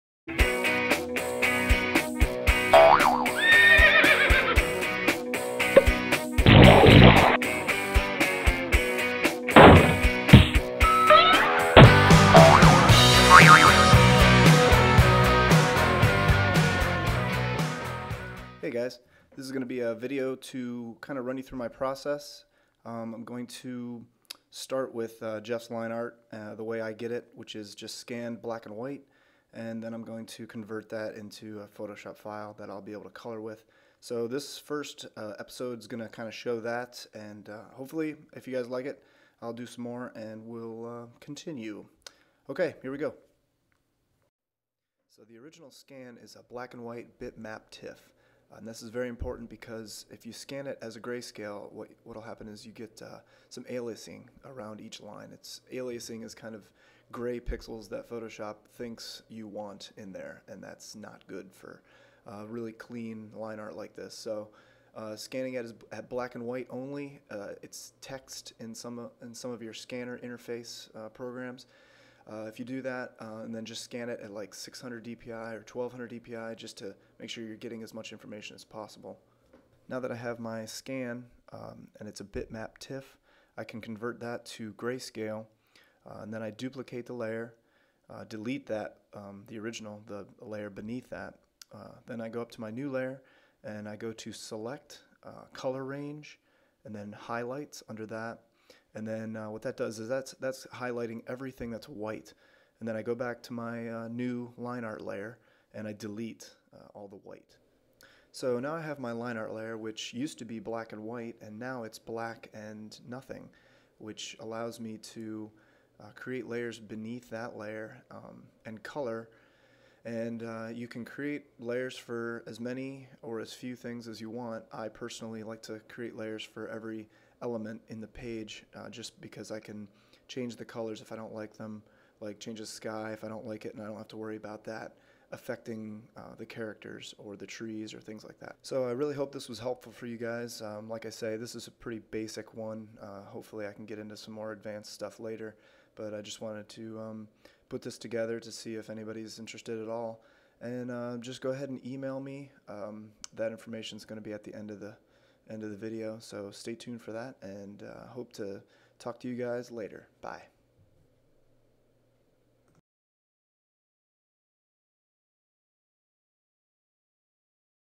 Coloring Comics the Steve Hamaker Way: Ep 1
Tutorial walkthrough and tricks for coloring comics using Photoshop.